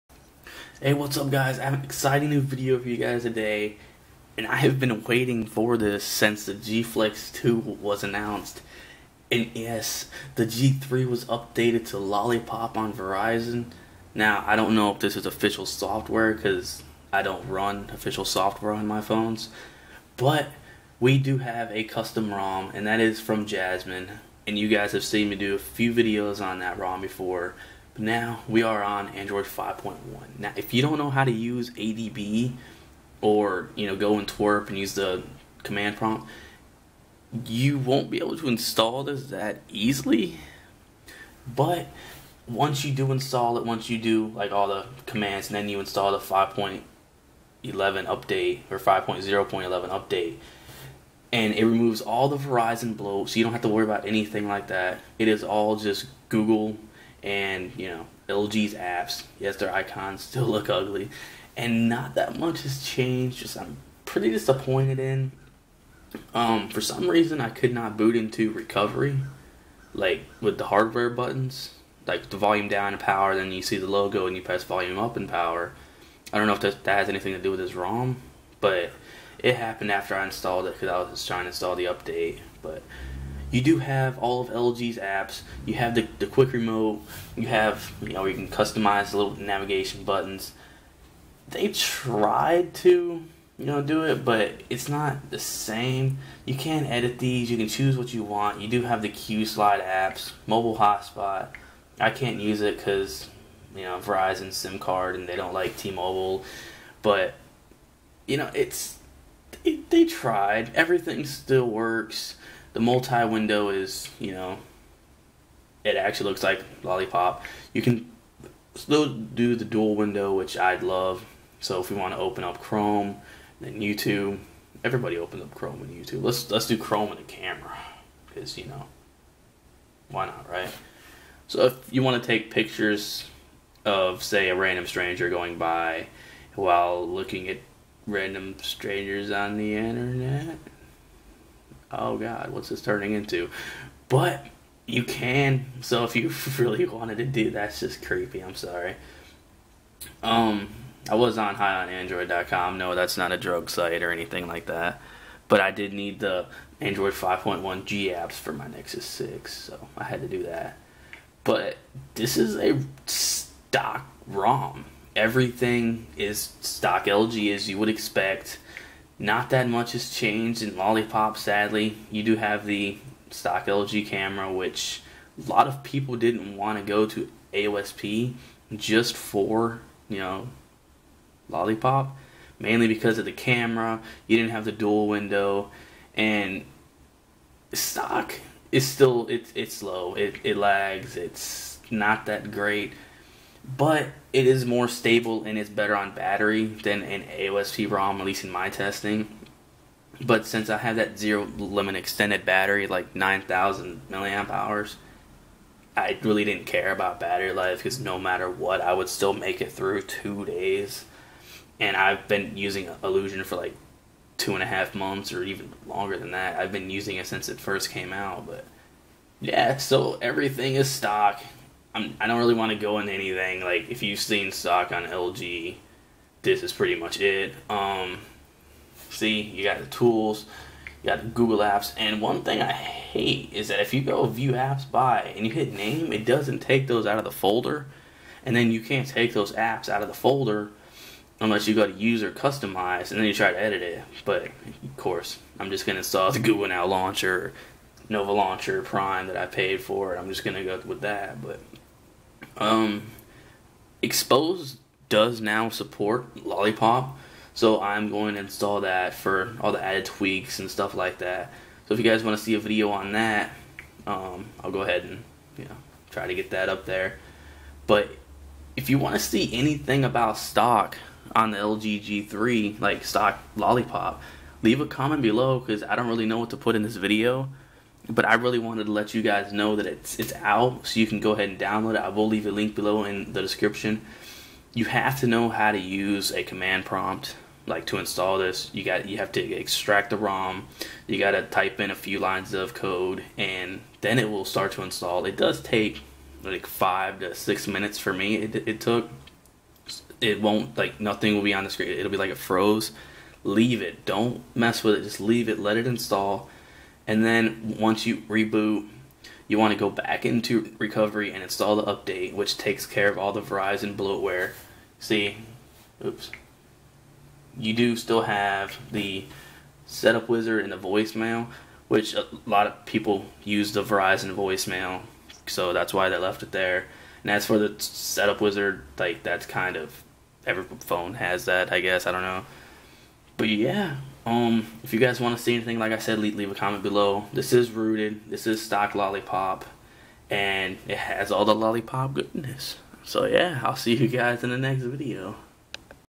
Quick Look At Android 5.0 From Jasmine ROM v5.0.11
This is LG's take on Android 5.0 Lollipop on the Verizon G3 VS985.

Phone not recognized by the computer? Install the drivers for your device:
Motorola: Ha!